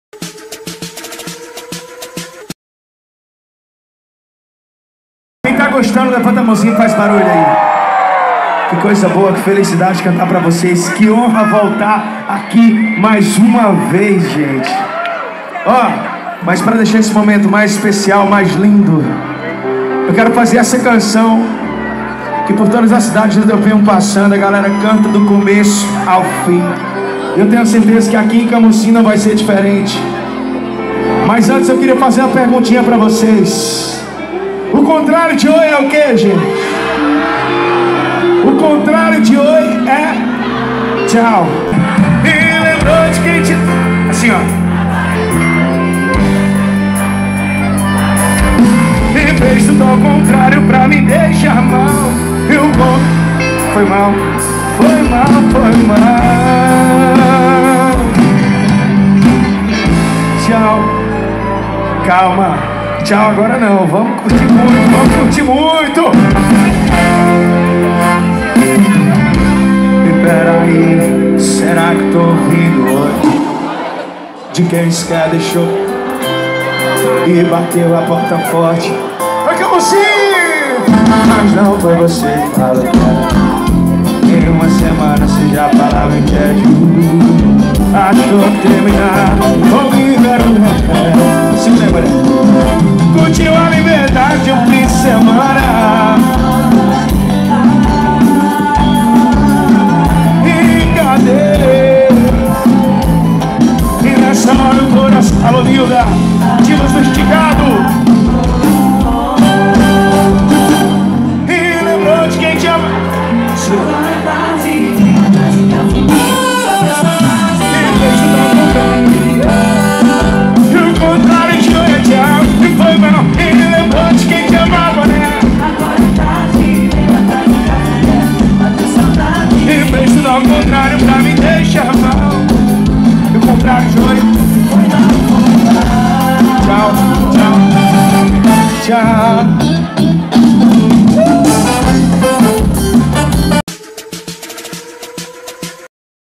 JONAS ESTICADO (contrário)
De volta a Camocim Jonas Esticado participou da terceira noite de carnaval em Camocim Ceará.
Os acontecimentos que foram destaques estão aqui no canal ACONTECE, seu resumo semanal de noticias.